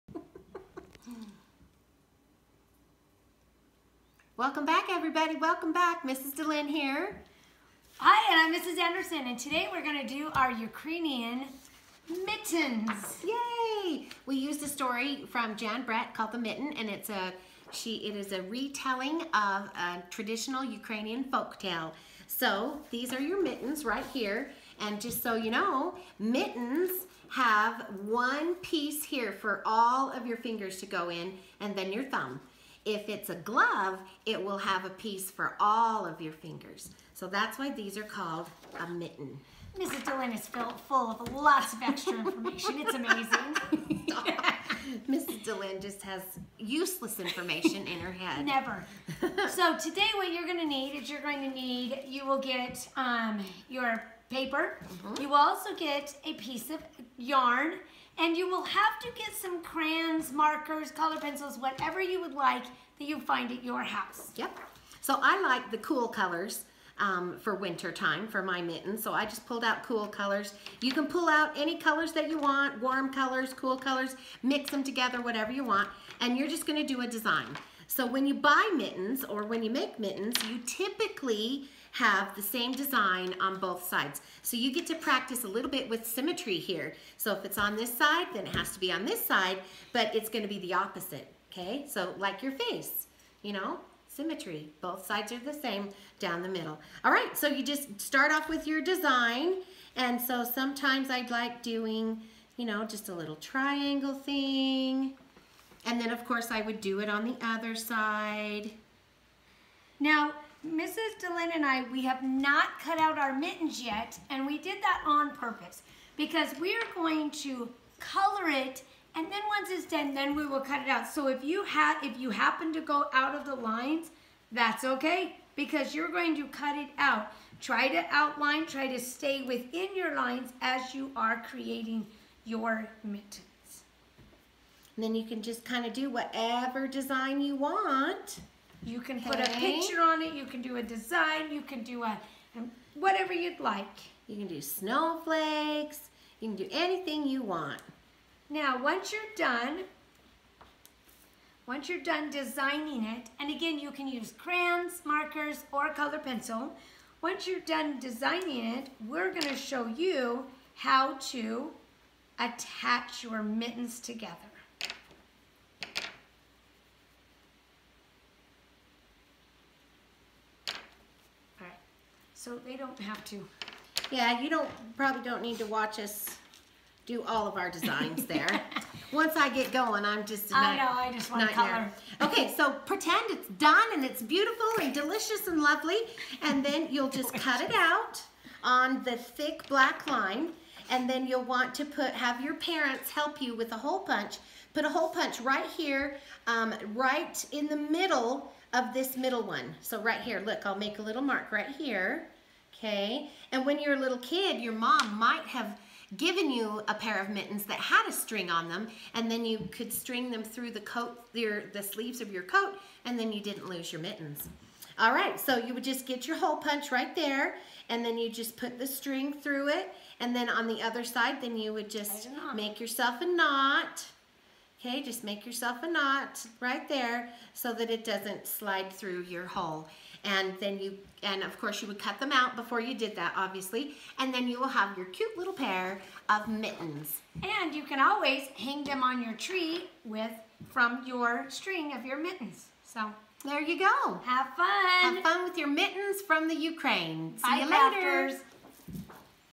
Ukraine -Mitten Craft
Winter Traditions Around the World
Mrs. Anderson and Mrs. Dehlin make a Ukrainian Mitten Craft.